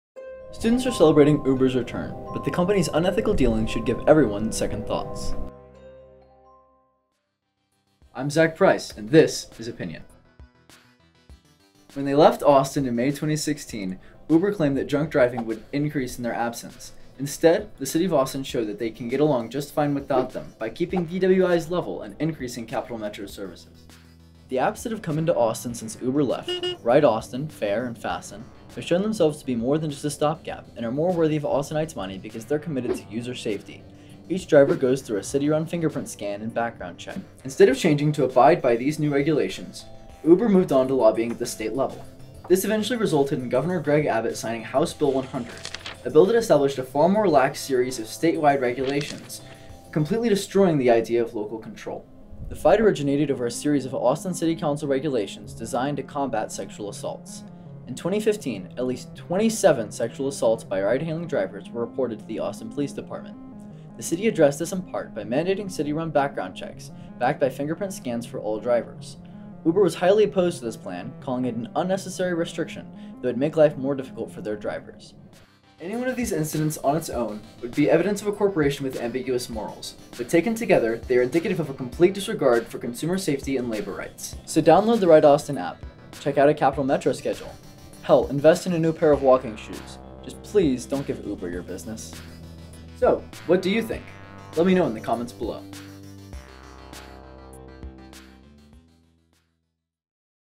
OPINION: Uber Back in Austin
Opinion Issue Columnist Zachary Price delves into Uber’s return to the Austin marketplace and the passing of House Bill 100.

Video by: Thomas Negrete (@thomasnegrete),
Courtney Joyner (@courtjoyner), & Peyton Young (peytonyoung37)
Reporter: Zachary Price (@Price_Zach)